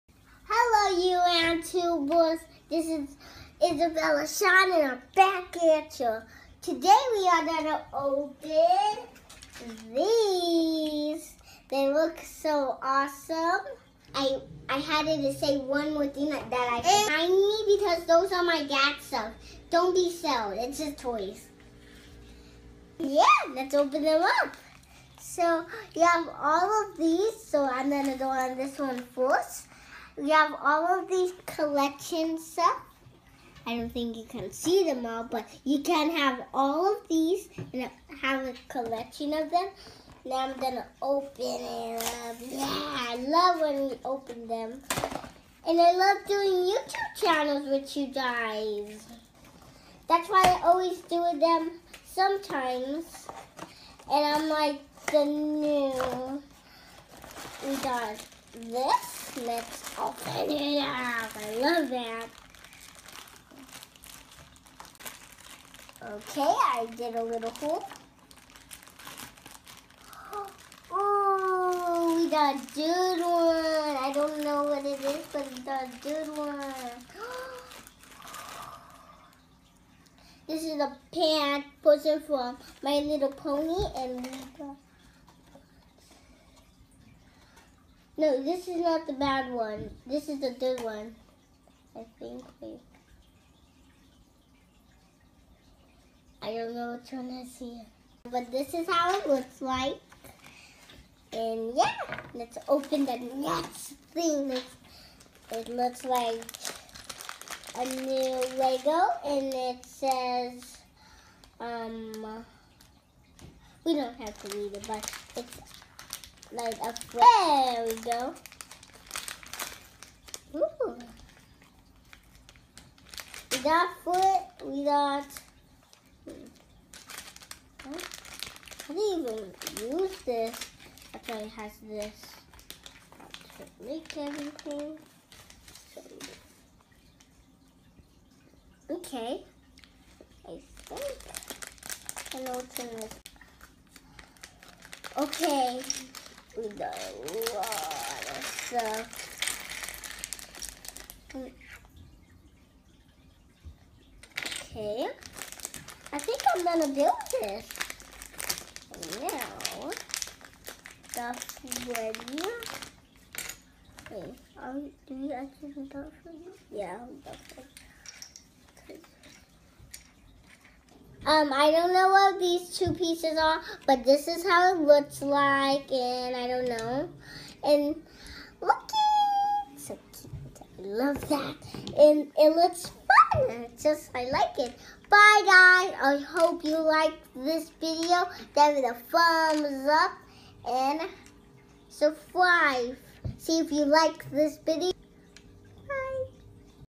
Blind bags
Unboxing My Little Pony and Five Nights at Freddy's